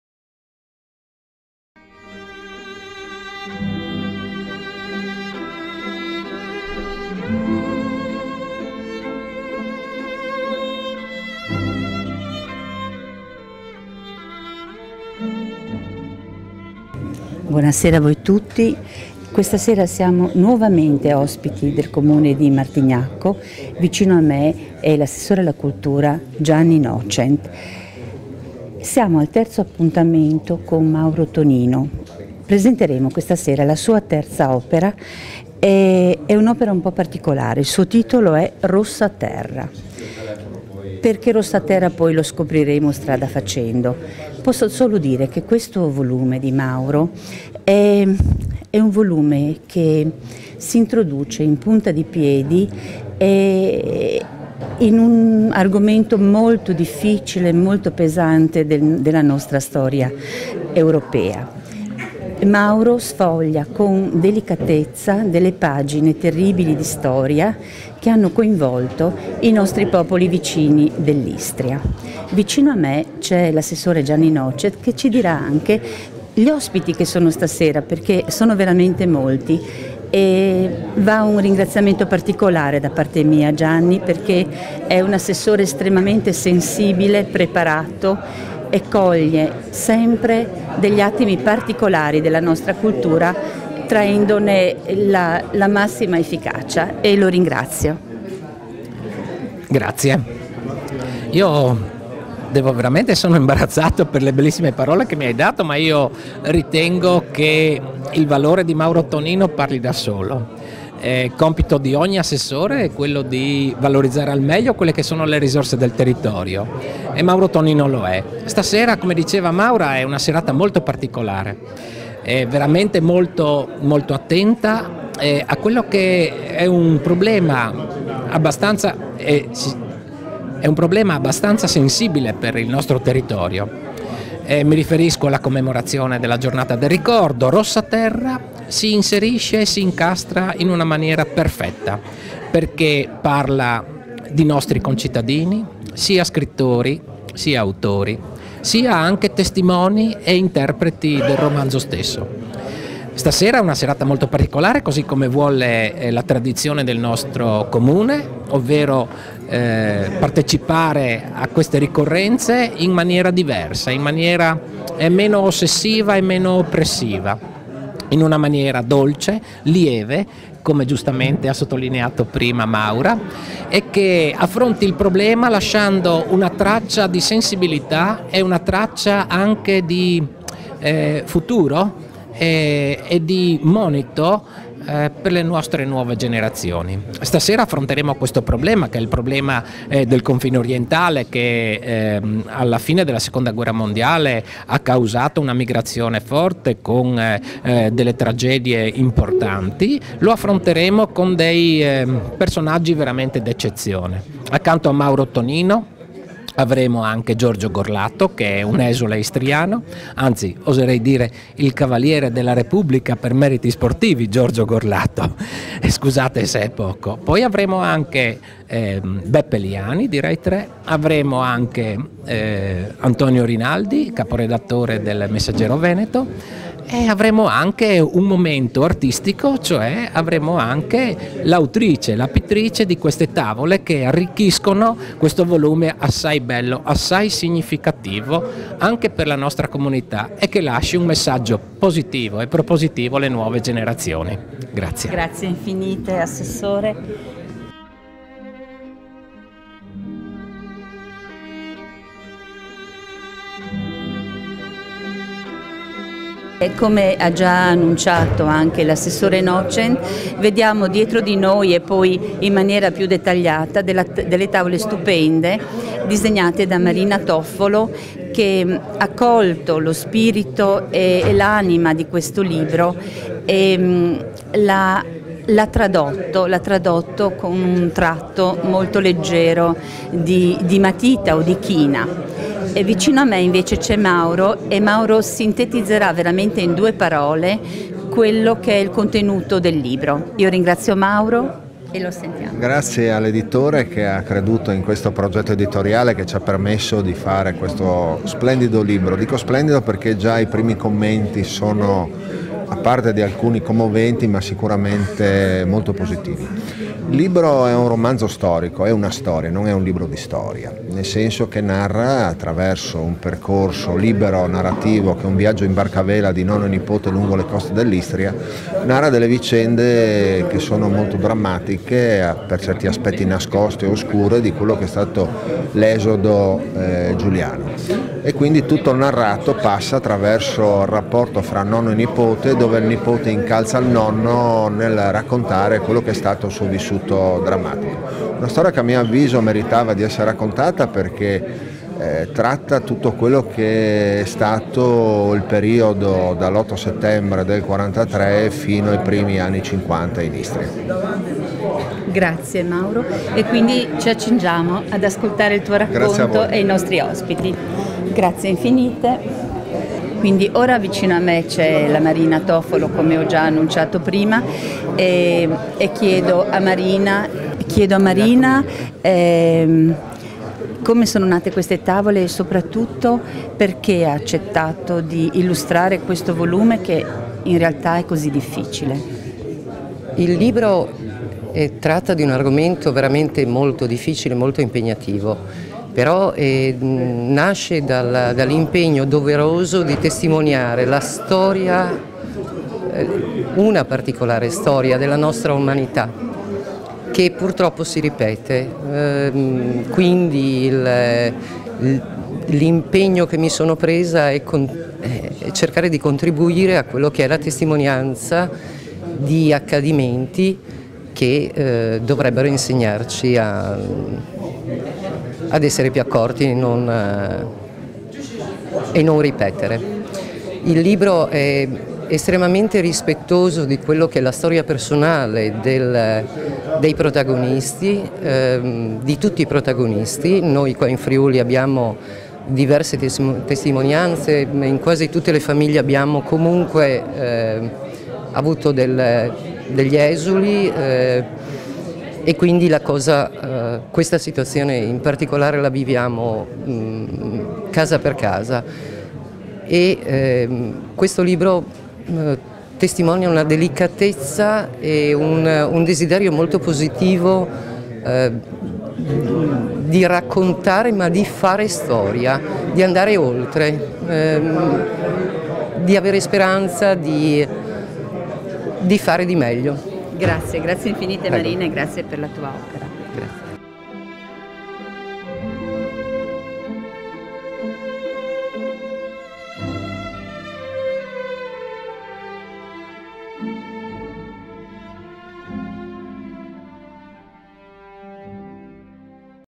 -La rossa terra- di Mauro Tonino, editrice Orto della Cultura
Martignacco : Presentazione libro "La rossa terra" di Mauro Tonino,  in collaborazione con la casa editrice Orto della Cultura di Pasian di Prato. A commentare l'opera ci saranno Giorgio Gorlato, esule istriano,Antonio Rinaldi, caporedattore del Messaggero Veneto, il giornalista Rai, Beppe Liani, mentre la presentazione è a cura di Marina Toffolo, che ha realizzato le tavole illustrative del romanzo. Sabato 16, invece, l'appuntamento è per le 11 in piazzale Martiri delle Foibe, dove sarà inaugurato un cippo commemorativo dei massacri delle foibe e dell'esodo giuliano-dal,  e scoperta una targa con iscrizione, che rinnova il ricordo della tragedia delle popolazioni vittime delle foibe e dell'esodo dalle loro terre.
Leonardo Editrice